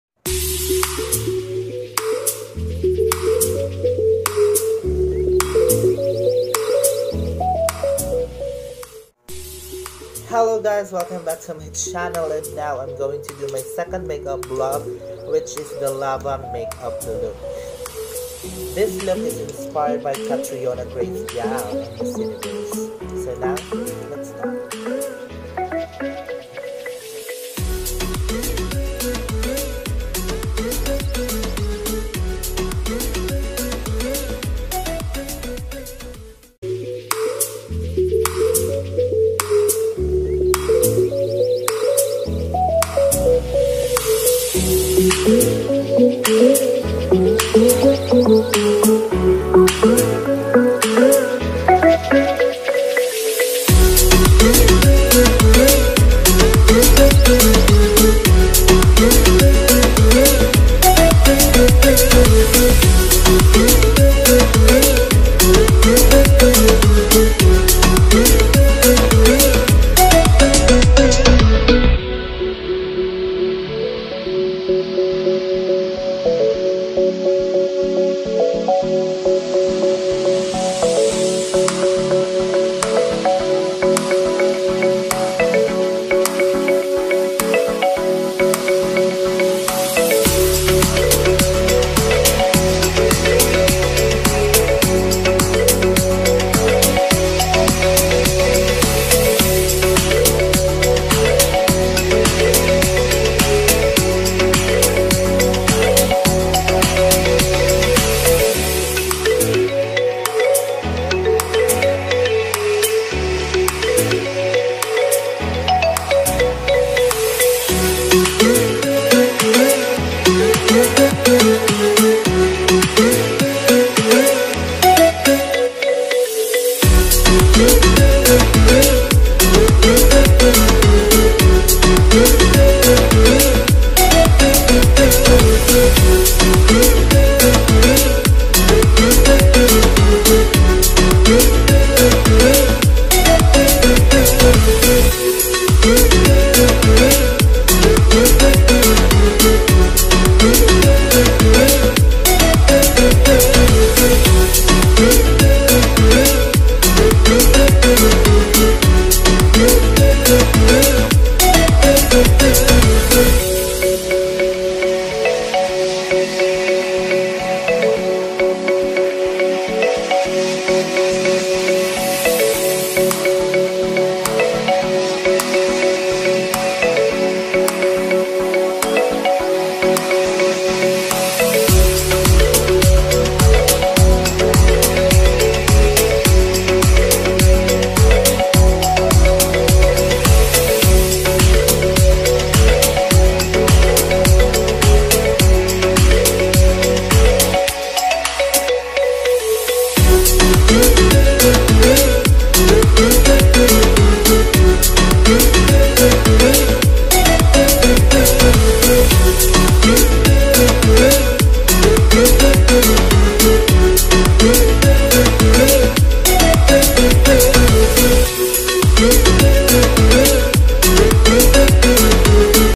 Makeup Vlog #2 (LAVA MAKEUP LOOK)
Hi Raffahssss!😊 This is my 2nd makeup vlog, inspired by the gown of our Miss U 2018.🔥🔥🔥🌋🌋🌋 Hope you LIKE it!😊😊😊 IF YOUR NEW TO MY CHANNEL please do SUBSCRIBE and hit notif bell 🔔 for you to update for my next makeup videos.😘😘😘